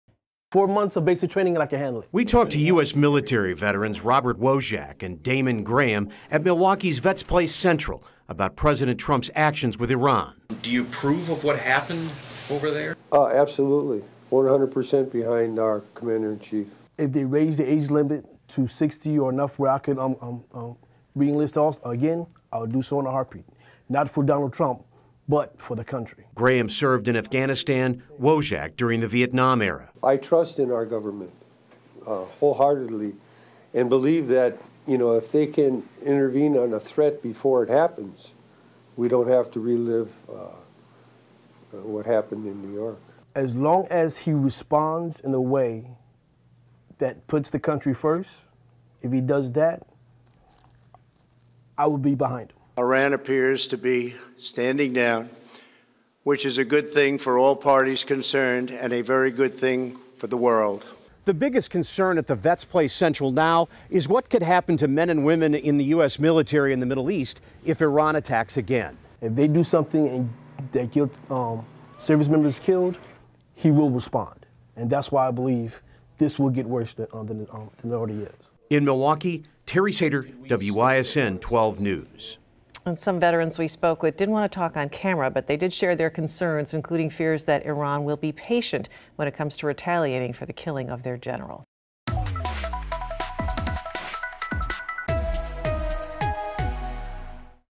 Veterans concerned for U.S. military members if Iran attacks again
Some veterans believe Iran will be patient when retaliating for the death of their general. Other veterans who WISN 12 News spoke with say they support whatever's best for the United States.